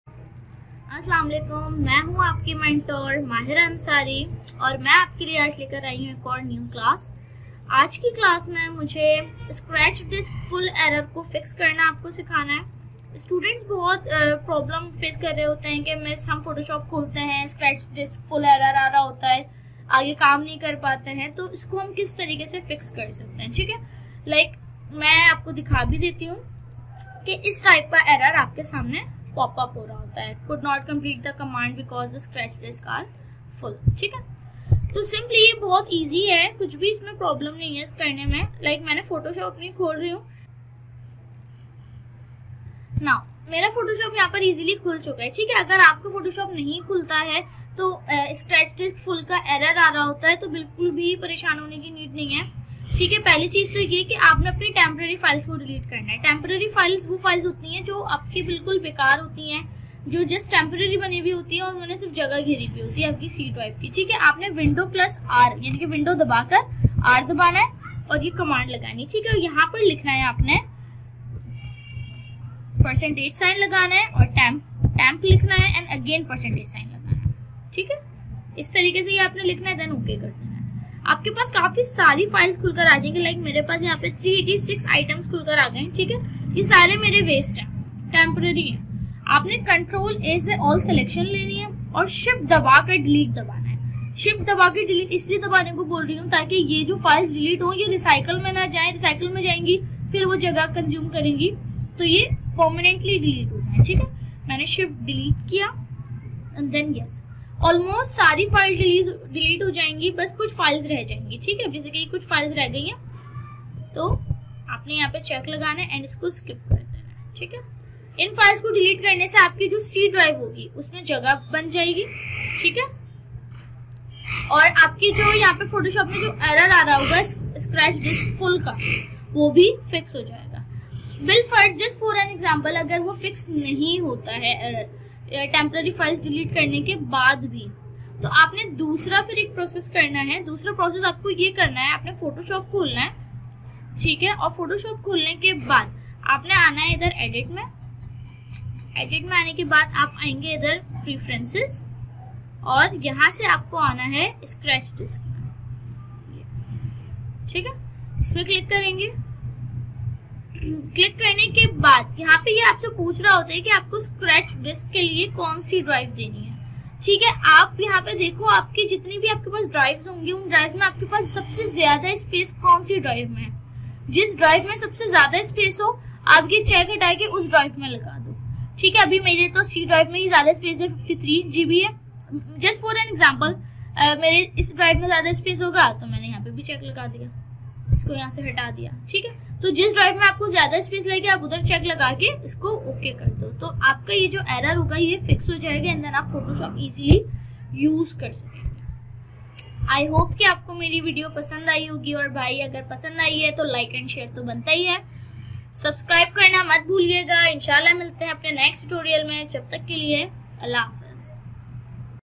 How to fix scratch disk full error in Photoshop | Urdu/Hindi Tutorial
In this tutorial, I will teach you how to How to fix scratch disk full error in Photoshop. Many students are facing these issues while using photoshop and ask me about how to fix them that's why I made this video. Now you can learn and fix the scratch disk full error in your photoshop software.